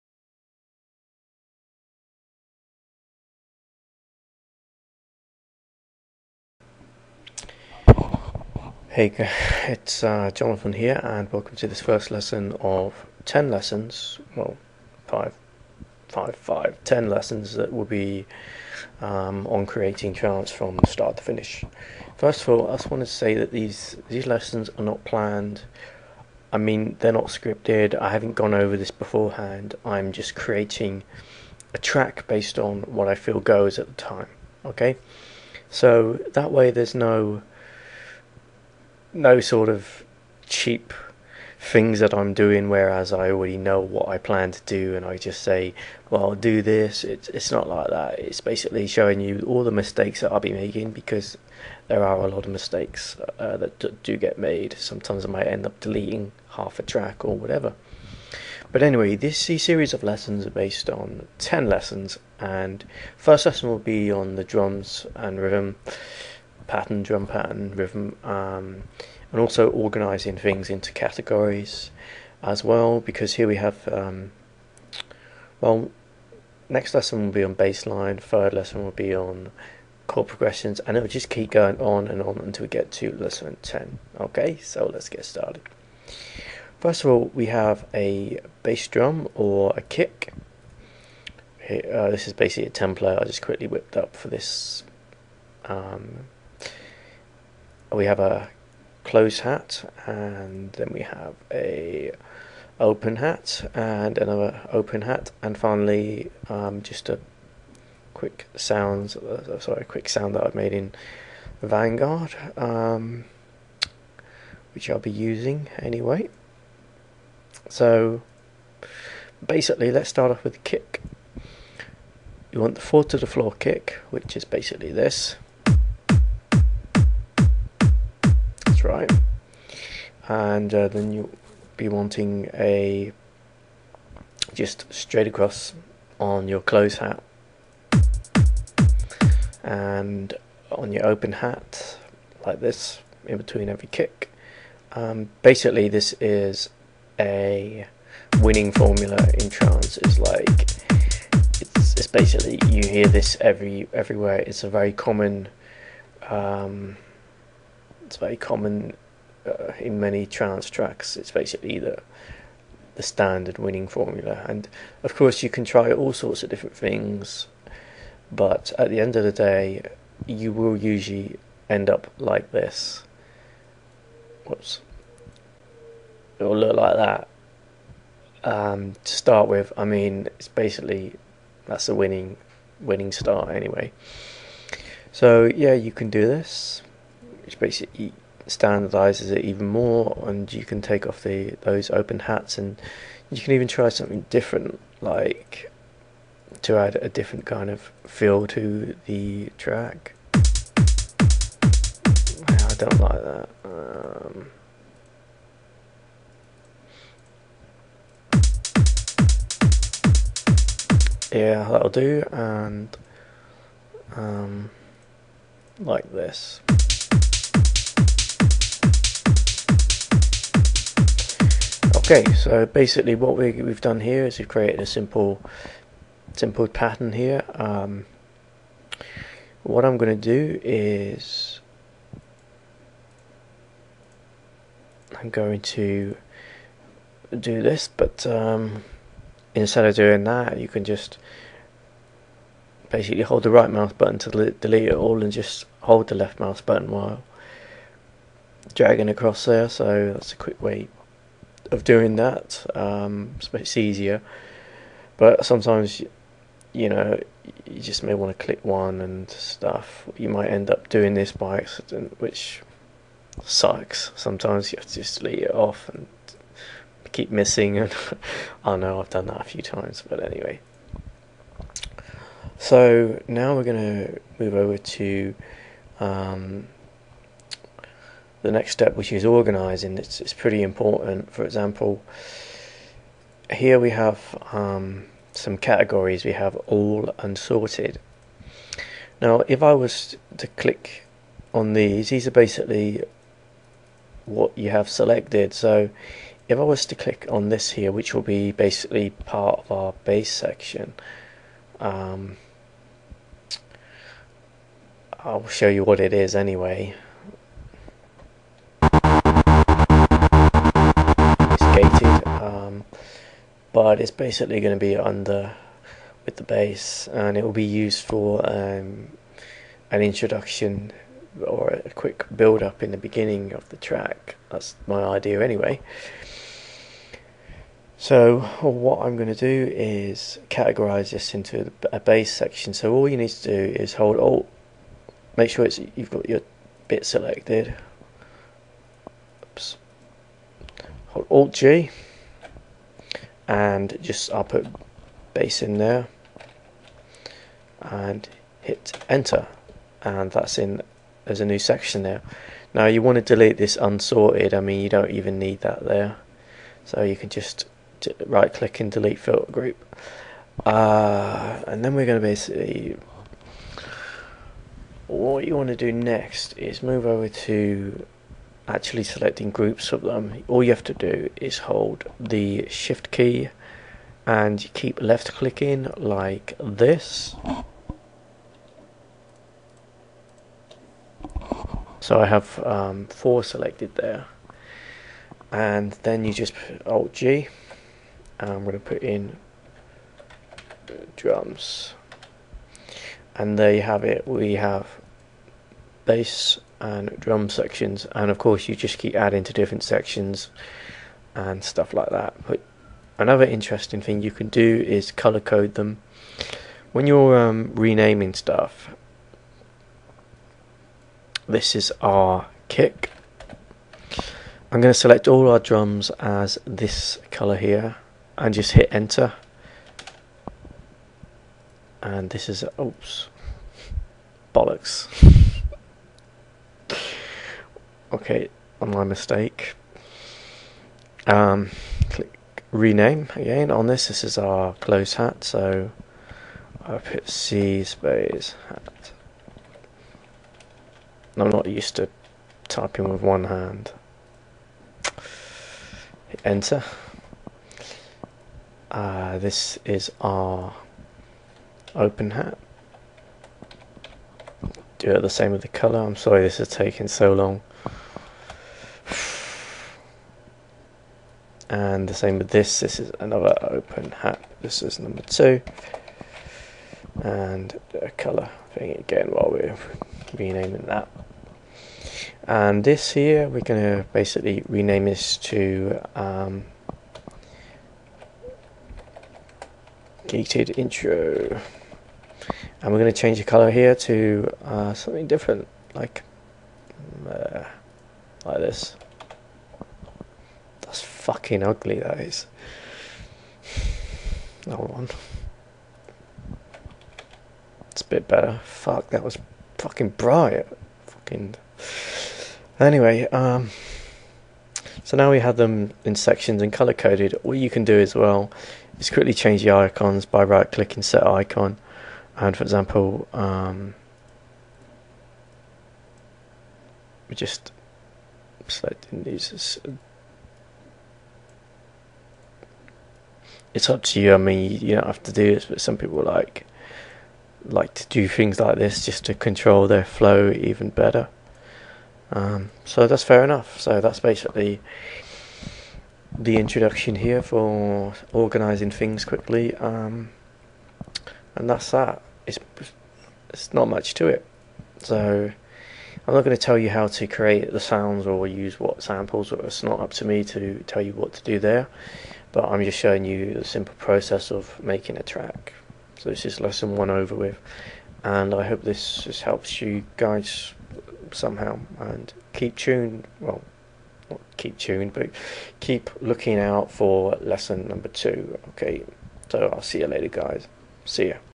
How to Produce Trance in FL Studio 9 - Lesson 1 (OUTDATED)
Welcome, here's my first lesson on getting the basic drum pattern down and organising things a little bit to give a more easier workflow. Hope it helps. This is part 1 of my 10 part lesson which we will be constructing a trance track from start to finish. There is no planned end result, so you're seeing the very basic production of how I make a track in FL Studio.